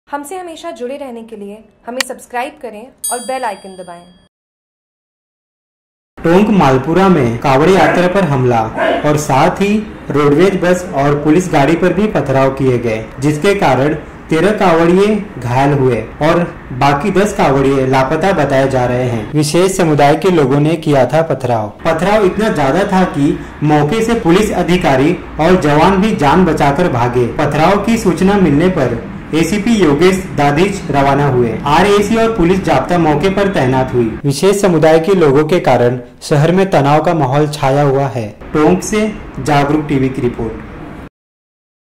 JAGRUK TV : टोंक : मालपुरा में कांवड़ यात्रा पर हमला
टोंक

मालपुरा में कांवड़ यात्रा पर हमला

रोडवेज बस,पुलिस गाड़ी पर भी पथराव

13 कांवड़िए हुए घायल,10बताए जा रहे लापता

समाज विशेष के लोगों ने किया पथराव

मौके से पुलिस अधिकारी-जवान भी जान बचाकर भागे

सूचना पर एसपी योगेश दाधीच हुए रवाना

आर ए सी और पुलिस जाप्ता मौके पर तैनात

टोडारायसिंह रोड़ का मामला

शहर में तनाव का माहोल